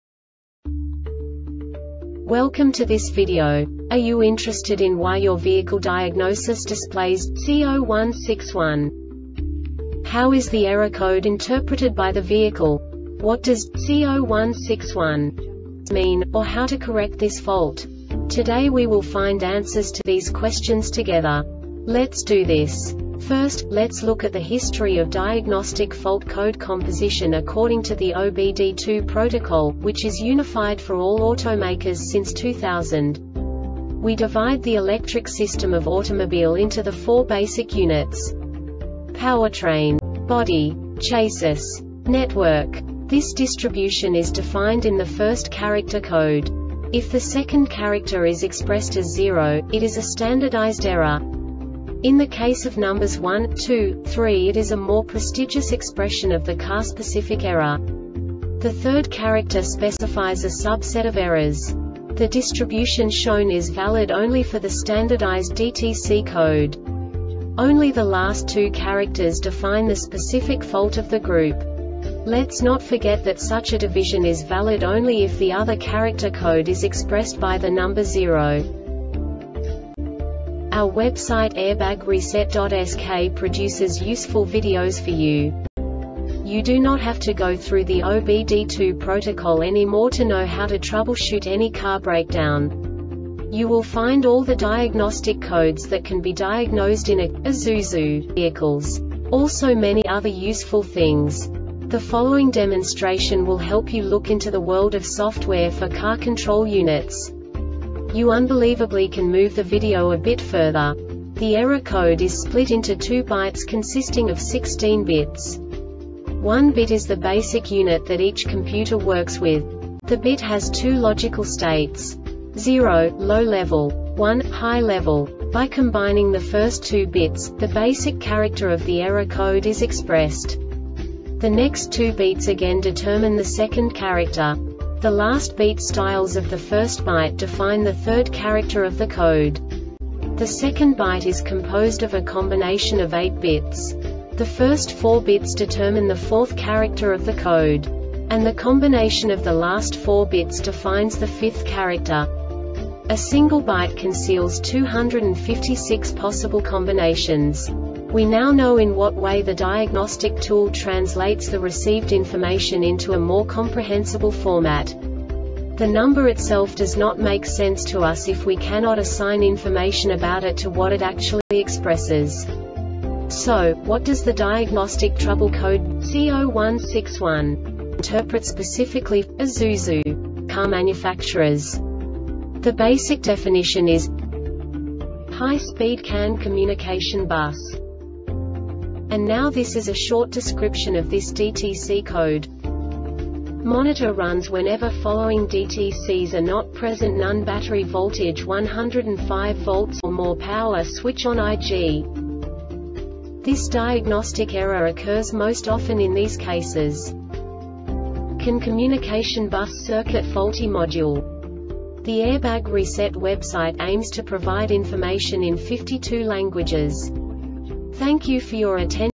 DTC Isuzu C0161 Short Explanation
Contents:
0:21 Basic DTC analysis according to OBD2 protocol standard.
1:48 Insight into programming
2:58 A diagnostically understandable description of the Diagnostic Trouble Code
3:05 Basic error definition

Make:
Isuzu
Code:
C0161
Definition:
High Speed CAN Communication Bus
Description: